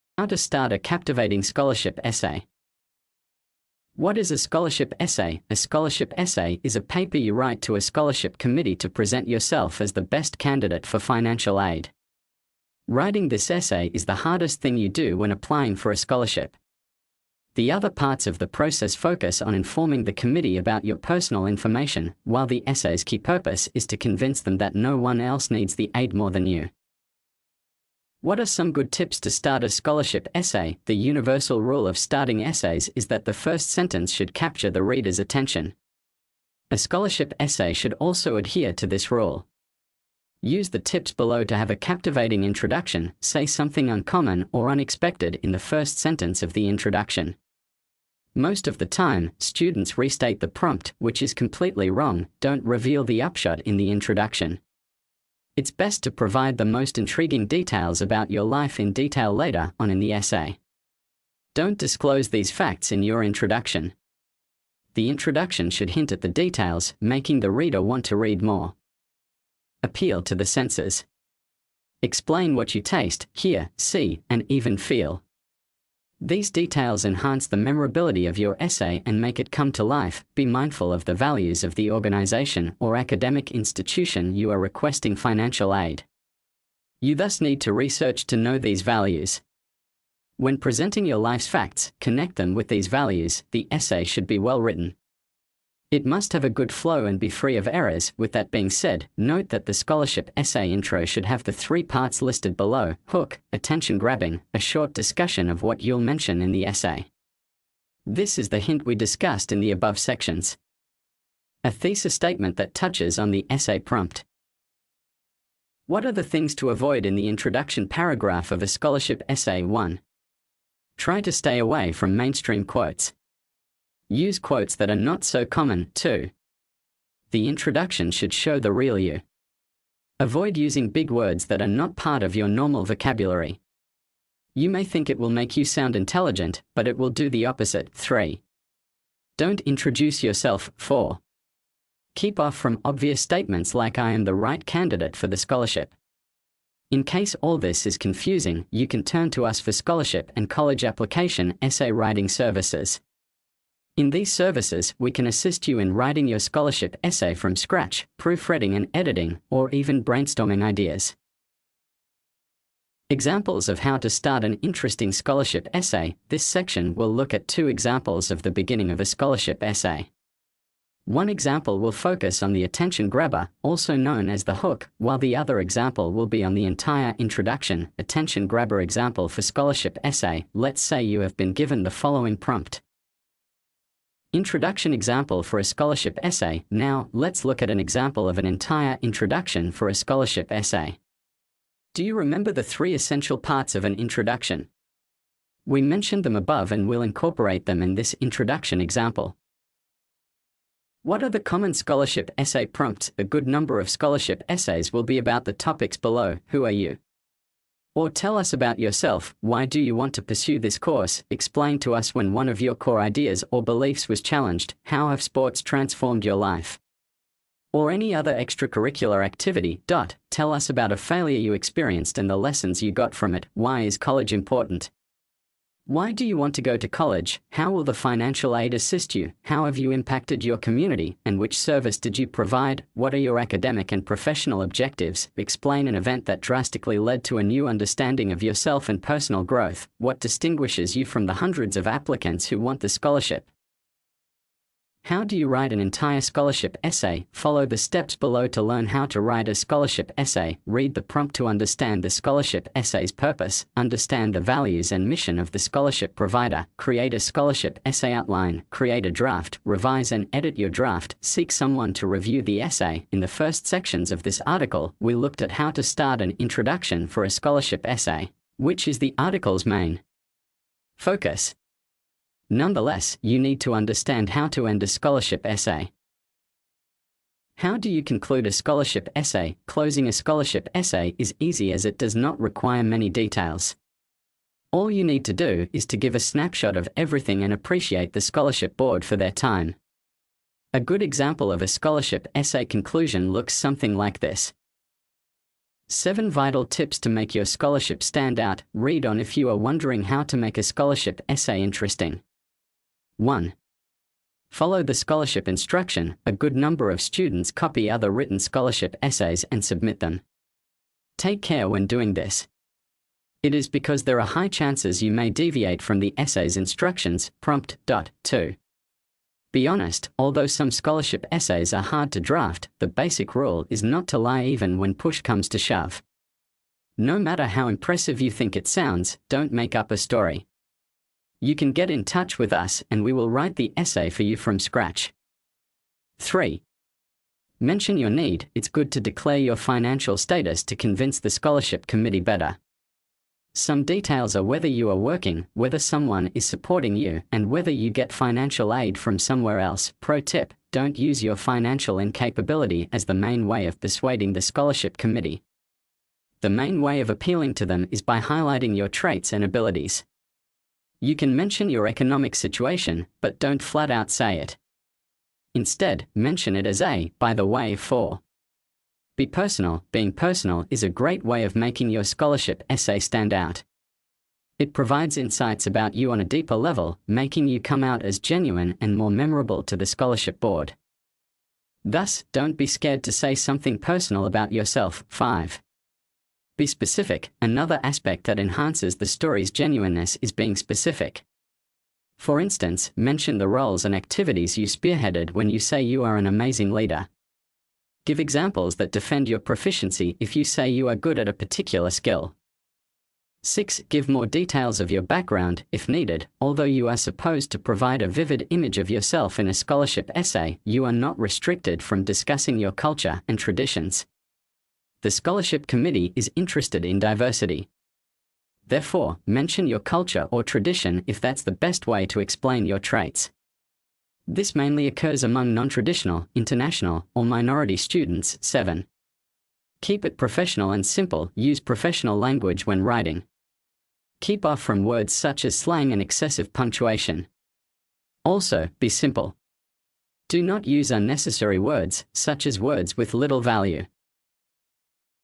How to Start a Captivating Scholarship Essay
Check out the updated version of How to Start a Captivating Scholarship Essay 🥇💸👨🏻‍🎓

Applying for scholarships can be competitive, but your scholarship essay is your chance to stand out from the crowd and secure that financial aid you deserve! In this video on how to start a captivating scholarship essay, we're here to help you kickstart your scholarship essay with an engaging and captivating introduction.

To get direct to the point you are more interested on use the time code below:

00:00:00 start
00:00:28 intro
00:01:55 Tips to Start A Scholarship Essay
00:02:40 Things to Avoid
00:03:03 Attention Grabber Introduction
00:03:21 Scholarship Introduction
00:04:04 Common scholarship prompts
00:04:37 how to write entire essay
00:04:55 How Do you Conclude
00:07:26 Tips to Make Outstanding Scholarship